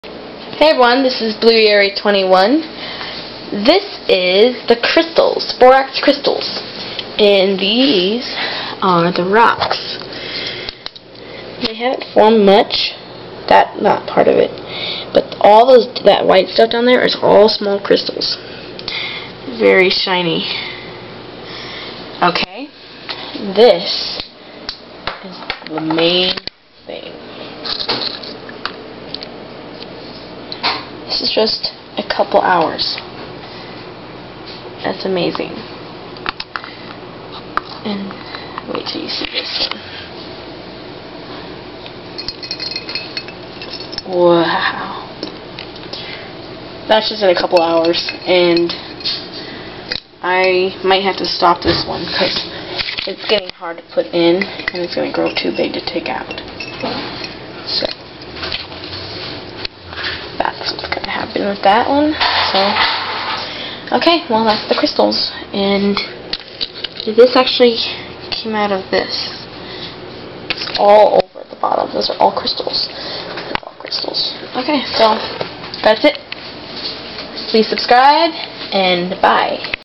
Borax Crystals!
Please Realize that I was younger then I am now when I made this video. Yes there are many ways of making crystals and I probably do not do them the Best possible way, but guess what... IT WORKED